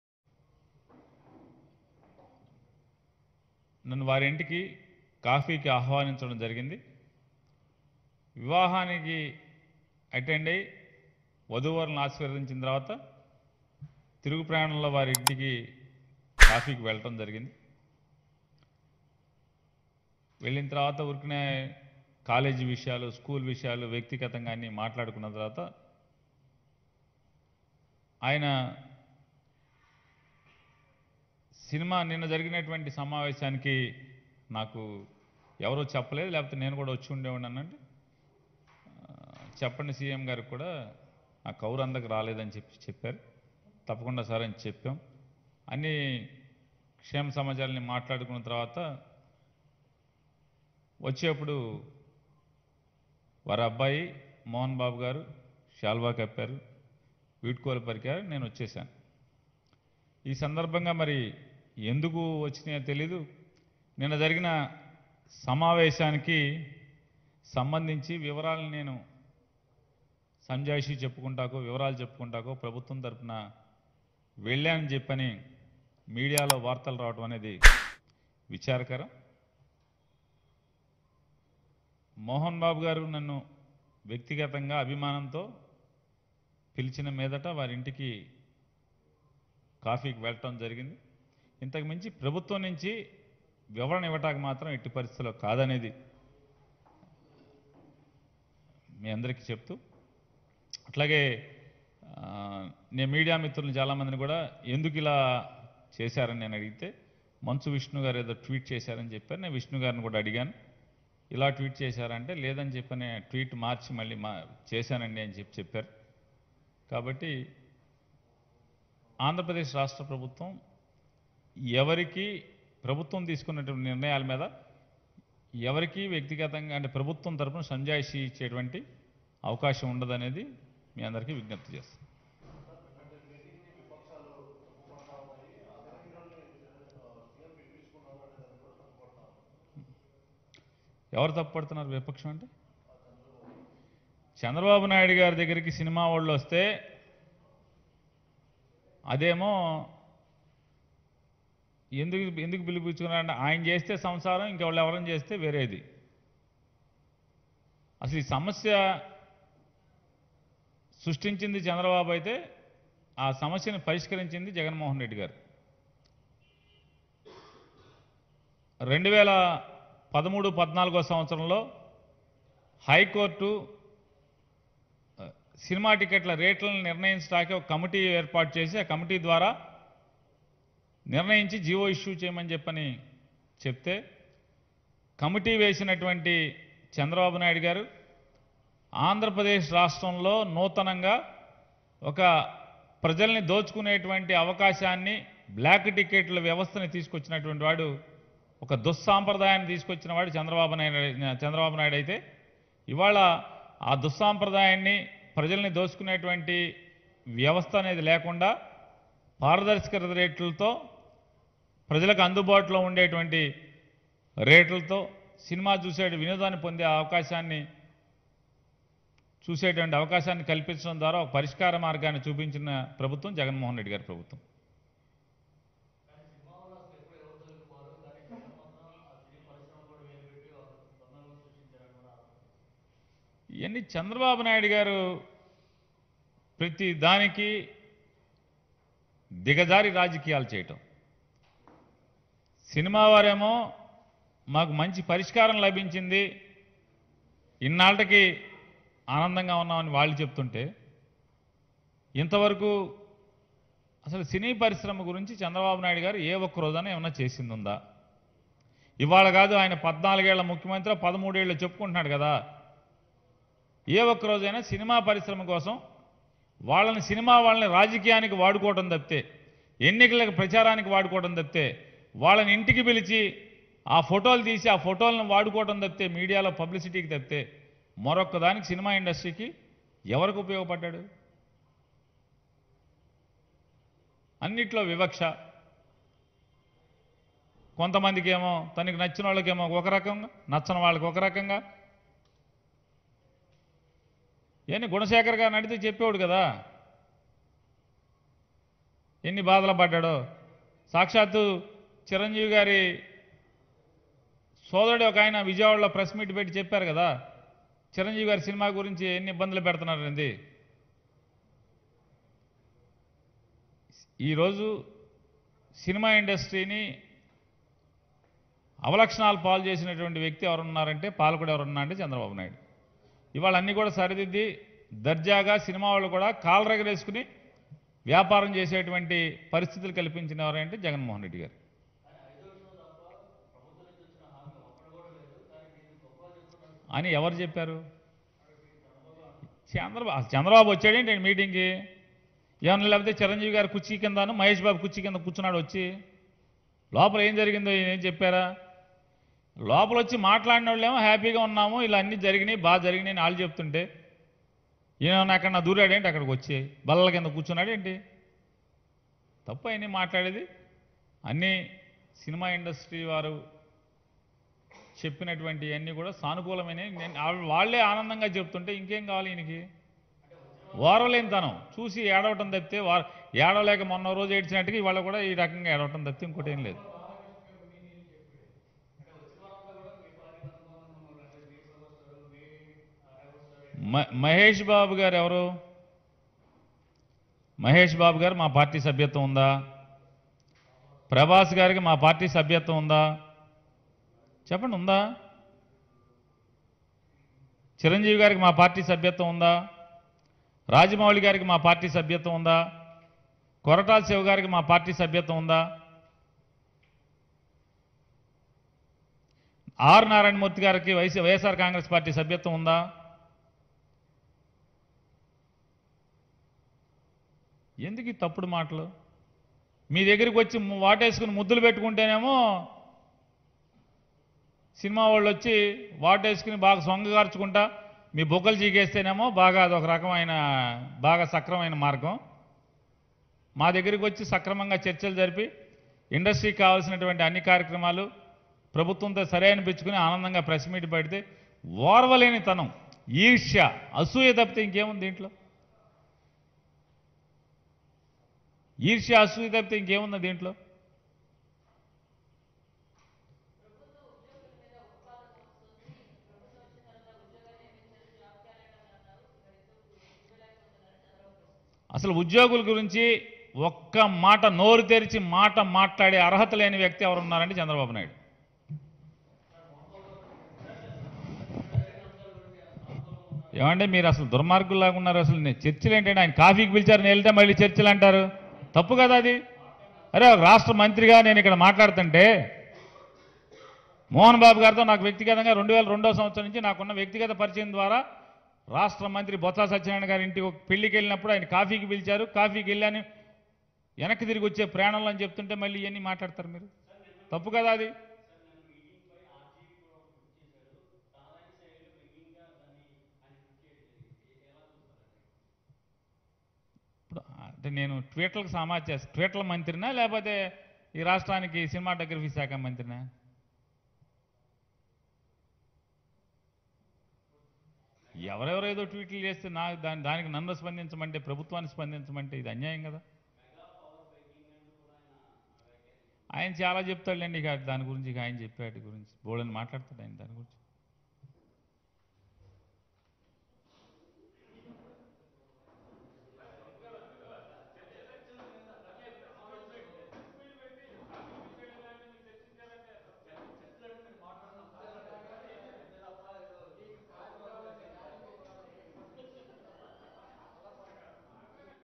Minister for I&PR and Transport & Cinematography Sri Perni Venkatramaiah (Nani) press meet
Twitter Minister for I&PR and Transport & Cinematography Sri Perni Venkatramaiah (Nani) press meet